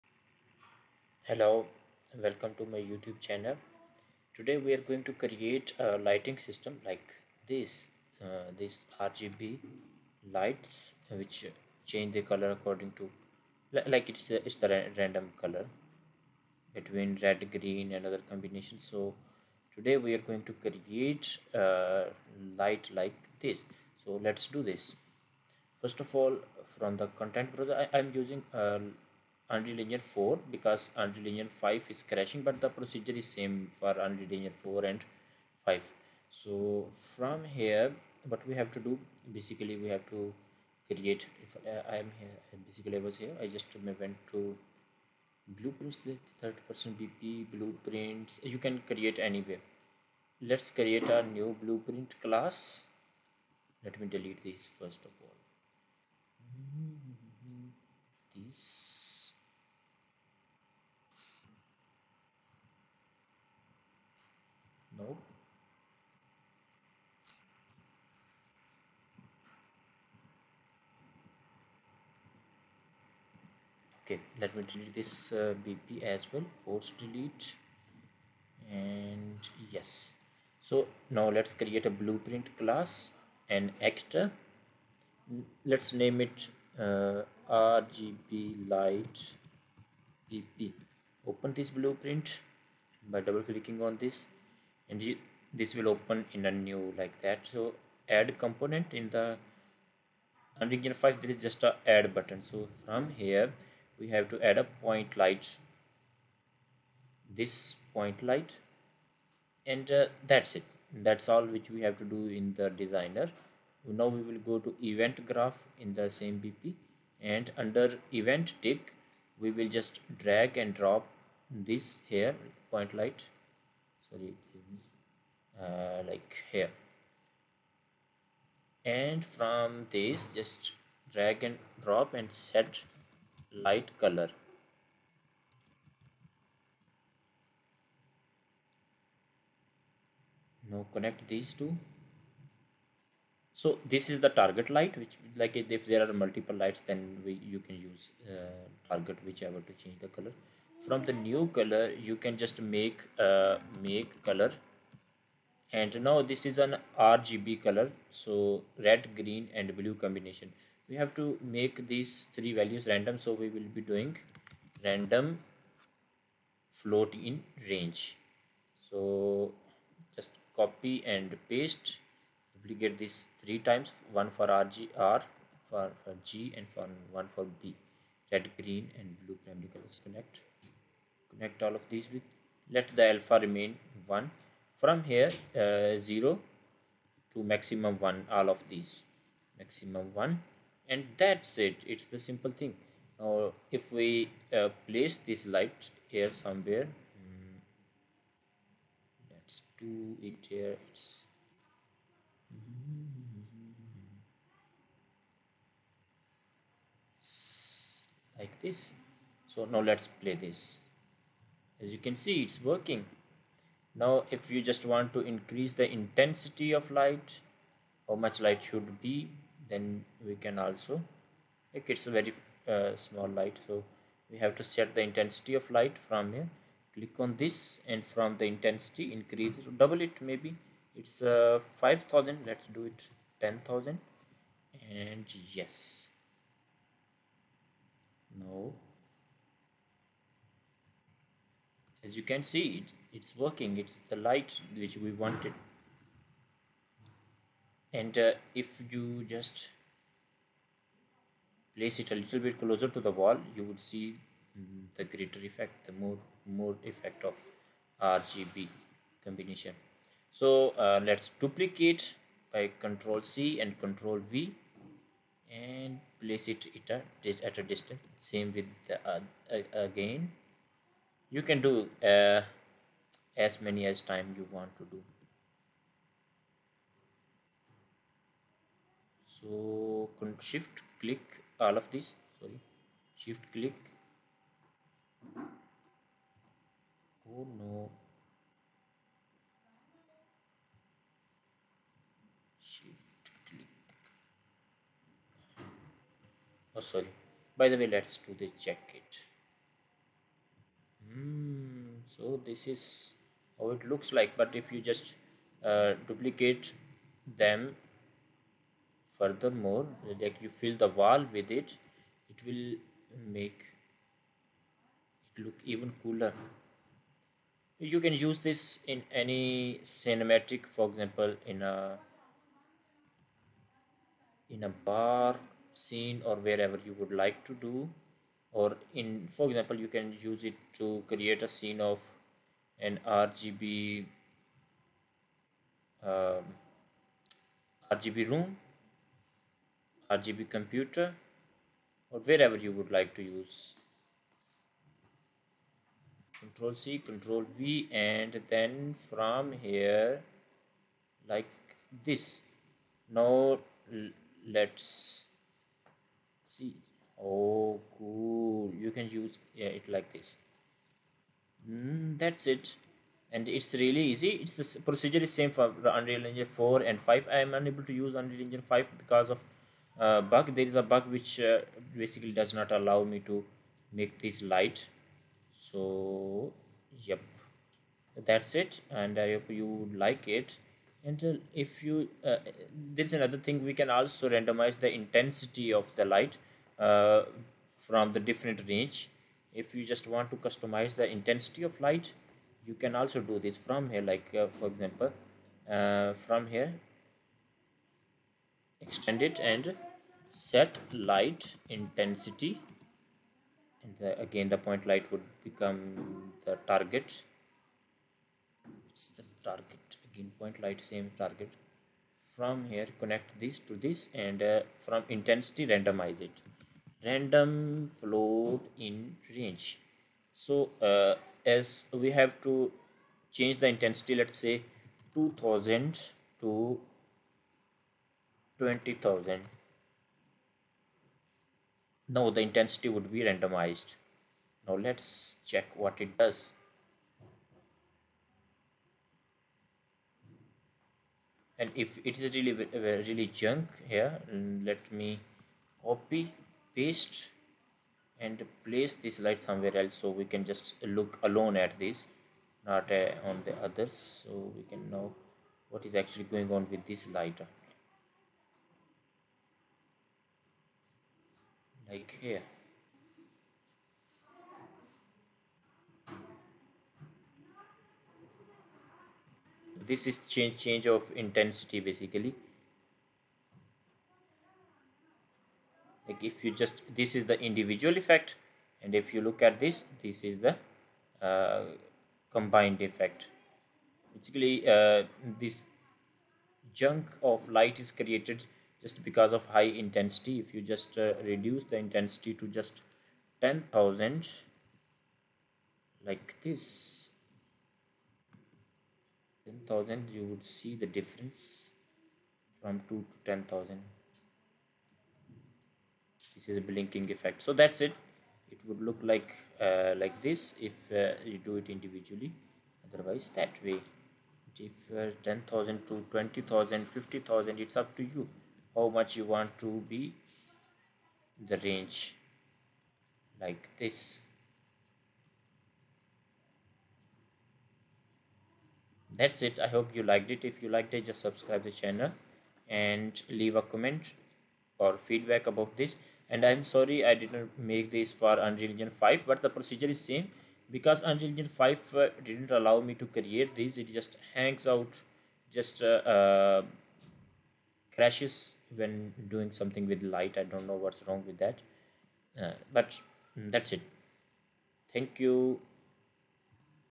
How to make RGB Light in Unreal Engine | Unreal Engine Tutorial
In this video, we are going to learn about creating RGB light which changes its color and intensity randomly.  You can use it in any game or scene and it is very easy to create.
Welcome to our Unreal Engine RGB light tutorial! In this video, we will be showing you how to create and customize RGB lights in the Unreal Engine.

RGB lights are a type of light that can be customized to display any color of the rainbow, giving you a lot of flexibility when it comes to lighting your scenes. We will start by showing you how to create an RGB light and how to adjust its color and intensity.

Next, we will demonstrate how to use the RGB curves feature to create dynamic, animated lighting effects. We will also show you how to use the color picker to precisely match the color of your lights to specific objects or materials in your scene.

By the end of this tutorial, you will have a solid understanding of how to work with RGB lights in the Unreal Engine and be able to create stunning, colorful lighting effects for your projects.